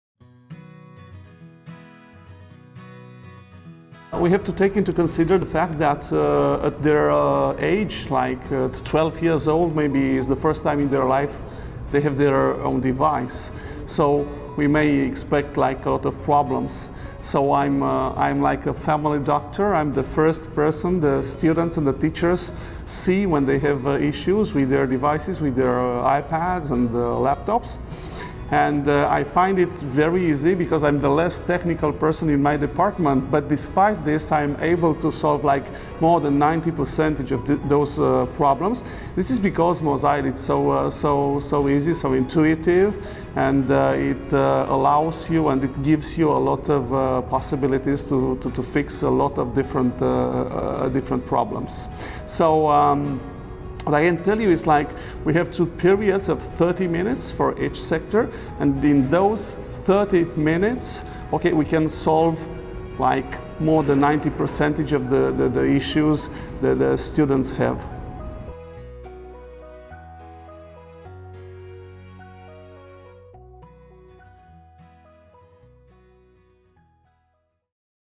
Les appareils Apple et Mosyle Manager 4 // Apple devices and Mosyle Manager 4
Découvrez comment Villa Maria transforme l’apprentissage avec les appareils Apple et Mosyle Manager (vidéos disponibles en anglais seulement). L’interface intuitive de Mosyle a à l’esprit la sécurité des élèves.
Discover how Villa Maria is transforming learning with Apple devices and Mosyle Manager. Mosyle’s intuitive interface has student safety in mind.